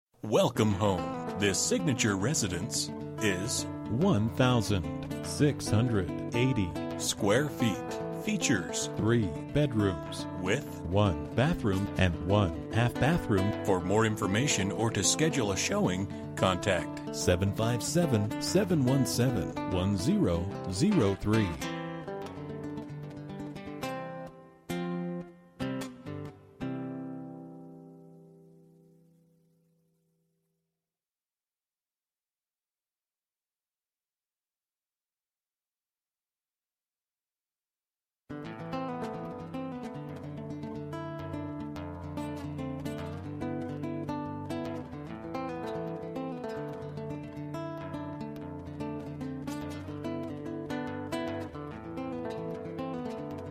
Property for Sale - 1314 W 27TH ST, Norfolk, VA 23508
3 bedroom 1.5 bath traditional 1680 sqft of living space in Lamberts Point. Nice sized fenced in rear yard with storage shed, large deck. Prior MLS sale updated in 2006... Must see to appreciate! Vacant go see and submit an offer today.

Bedrooms: 3
Bathrooms: 1 full and 1 half Bathrooms
Square Feet: 1680
Price: $89,900